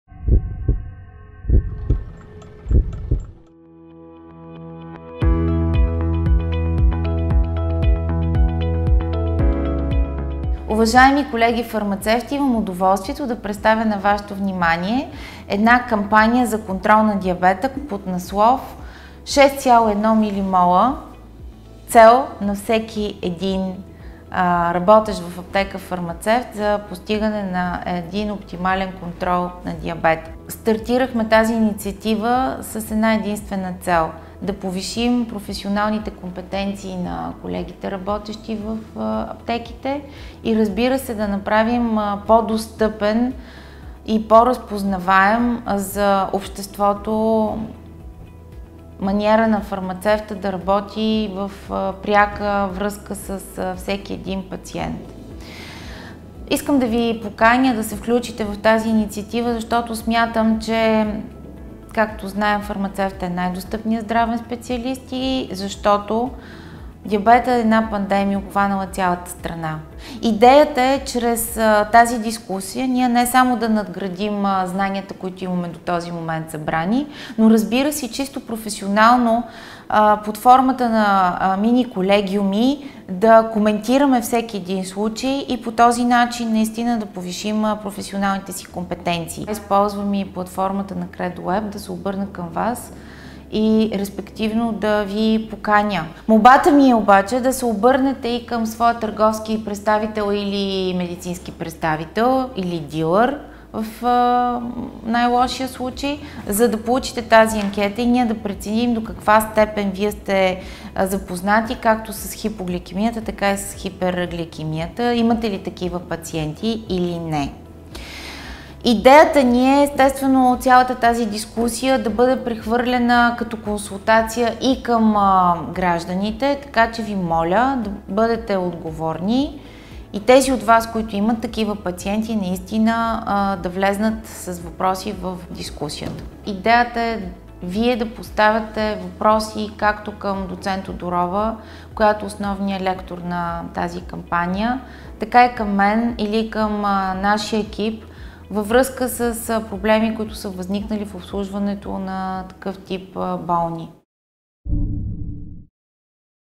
Ролята на фармацевта в здравните грижи за пациенти със захарен диабет
Маг. фармацевт Аделина Любенова